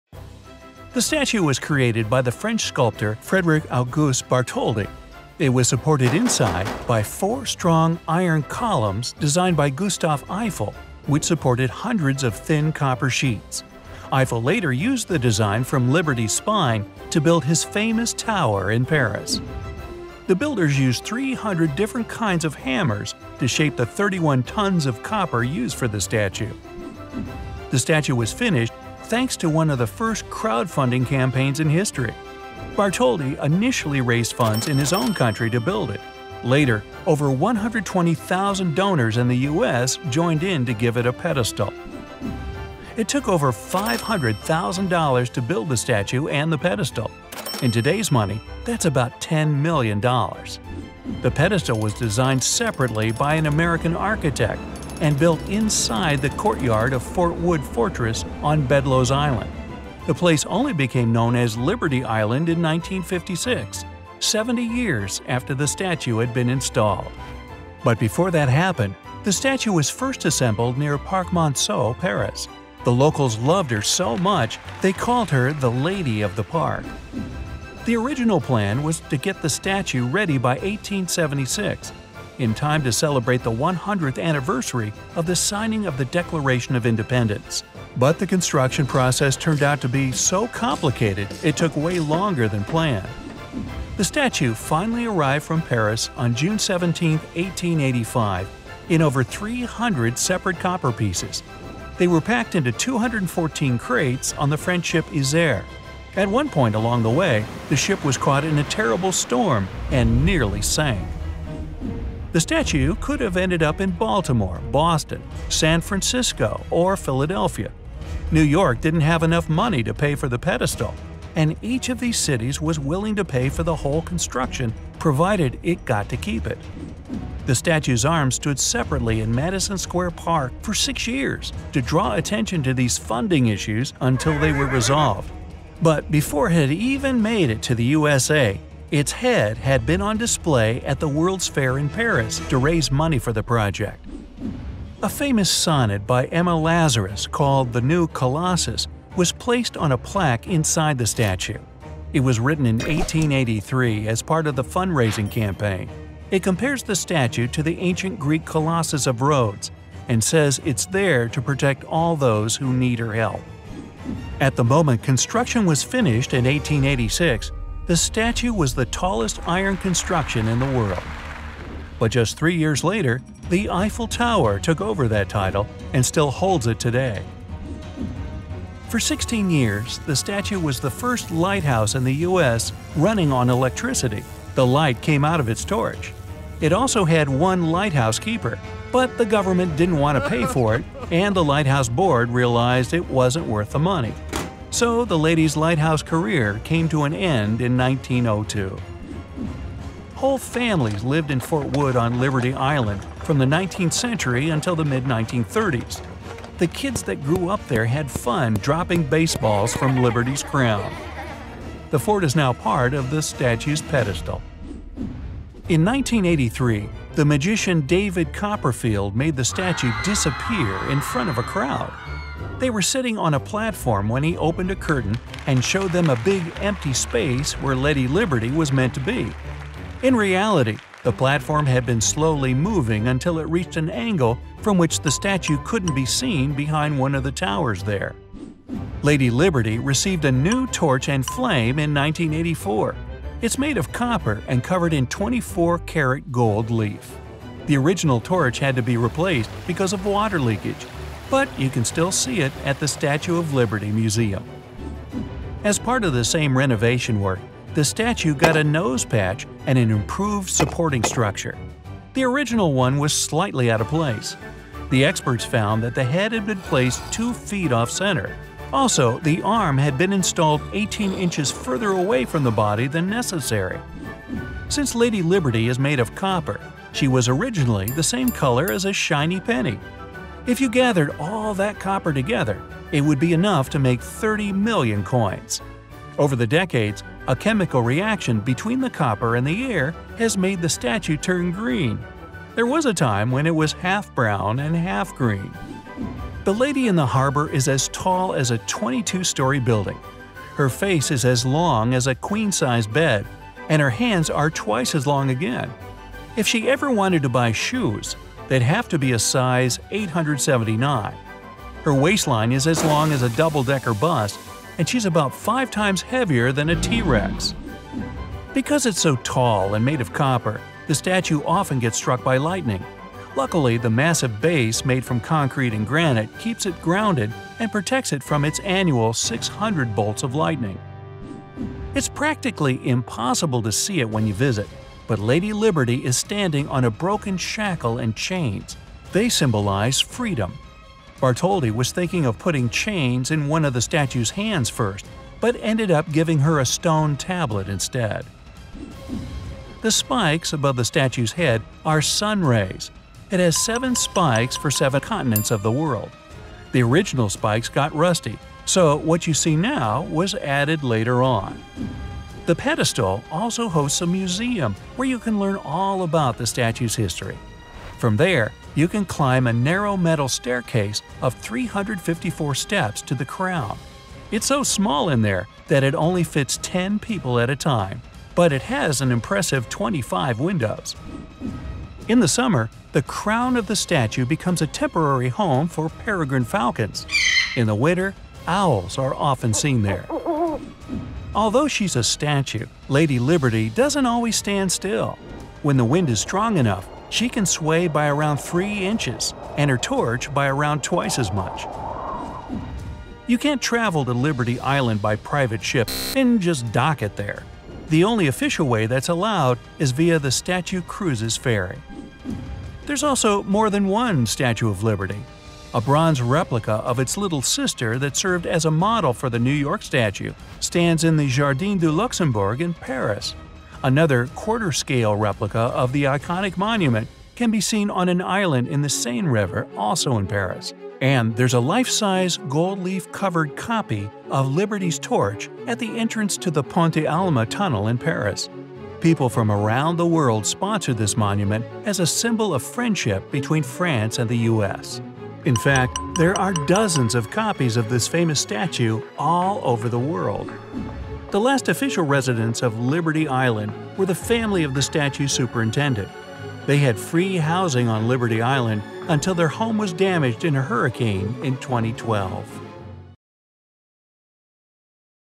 The Statue of Liberty Disappeared Once And Other Secrets You Didn’t Know
The Statue of Liberty was created by the French sculptor Frédéric Auguste Bartholdi. It took over $500,000 to build the Statue and the pedestal. In today’s money that’s about $10,000,000. The original plan was to get it ready by 1876, but the construction process turned out to be so complicated it took way longer than planned. The Statue arrived from France on June 17, 1885, in over 300 separate copper pieces.

Btw, did you know that the Statue of Liberty nearly didn’t get built, and it could have ended up in Boston? In 1983, it disappeared for a few moments. And it hasn’t always been green. And although she’s a statue, Lady Liberty doesn’t always stand still! Let’s take a look at the secrets of the most popular landmark in the world. Here are a few interesting facts you may not have known about the Statue of Liberty.

Inside the Statue of Liberty crown, 1980: By Rick Dikeman (Rdikeman),
A photo of a natural heritage site in Bangladesh: By Mosharaf Hossain Ce,
A bronze replica of the Statue of Liberty, found within Jardin du Luxembourg, Paris: By Eutouring,
The brass monument plaque originally affixed to the base of the Statue of Liberty: By Threefingered,